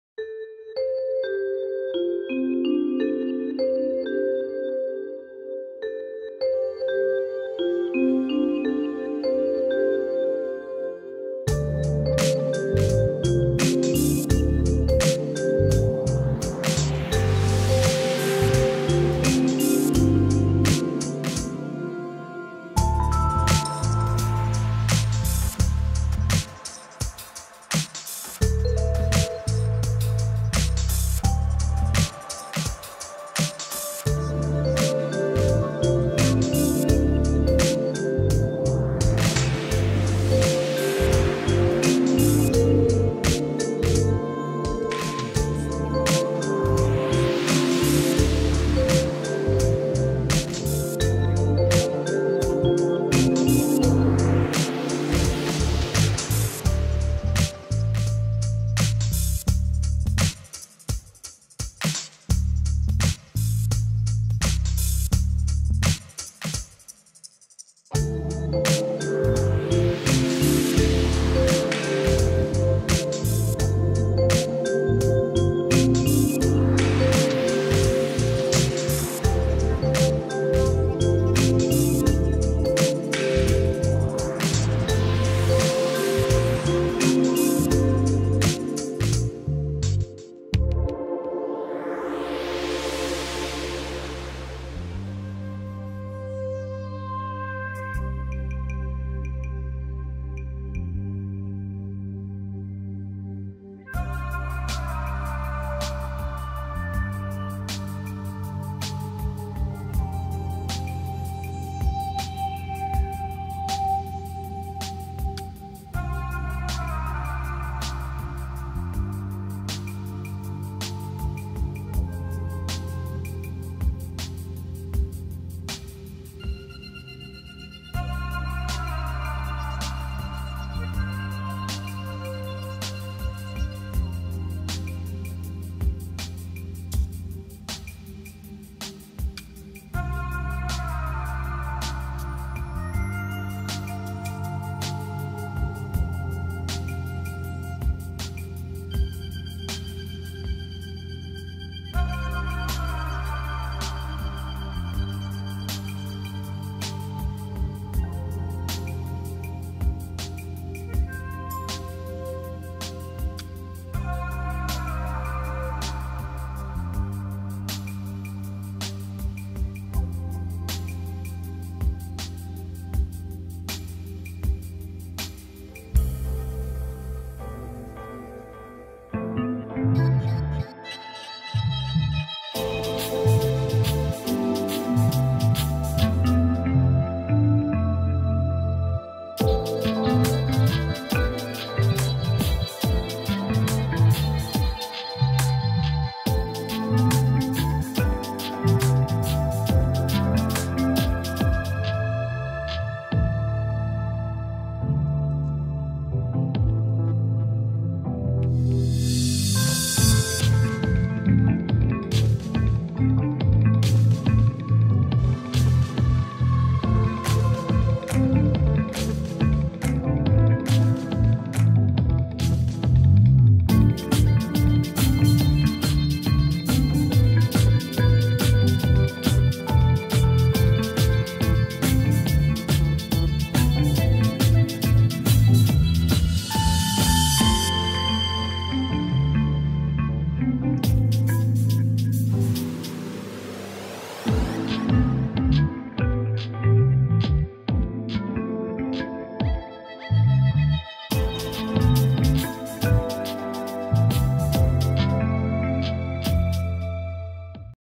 Iplehouse Carina being a Lingerie Model - video reel
Iplehouse FID Carina is such a gorgeous sculpt. She's like a real supermodel.
I decided to make another video reel whilst taking some gorgeous pictures. It would've definitely been a lot easier if she were better strung.